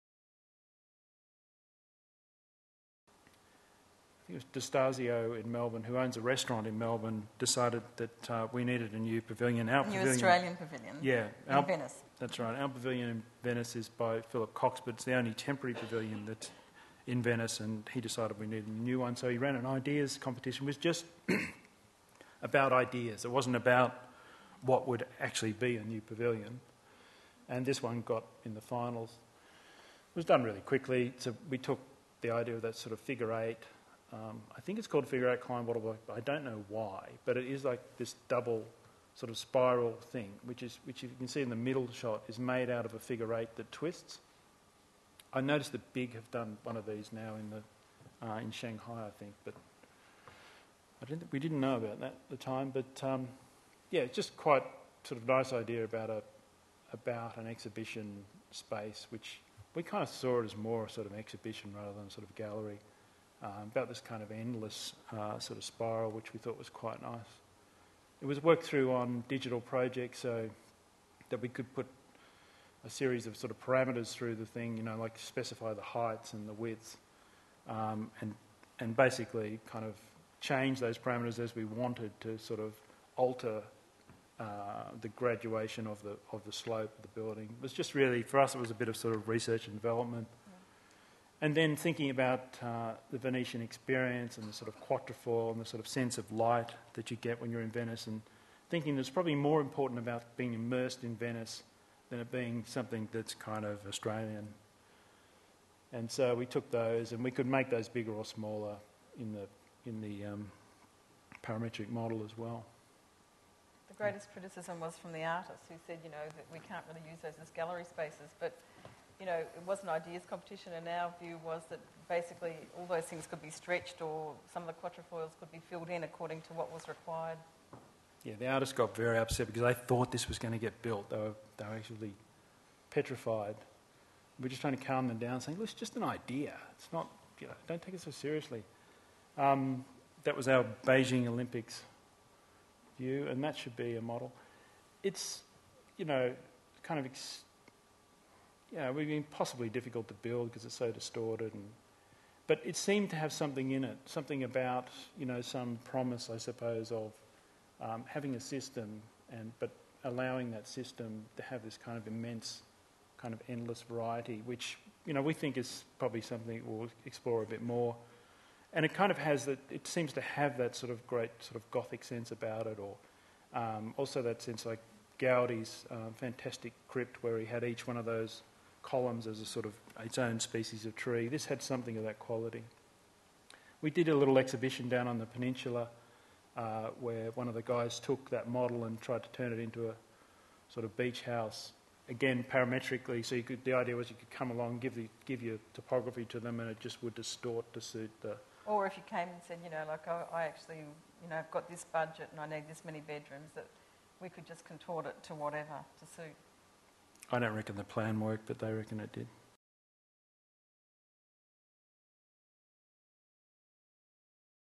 McBride Charles Ryan Lecture 5/12: The Venice Pavilion
Rob McBride and Debbie-Lyn Ryan of Melbourne's McBride Charles Ryan.

Part of Dulux New Zealand's Lecture Series filmed 15 September 2009 at Raye Freedman Arts Centre in Auckland.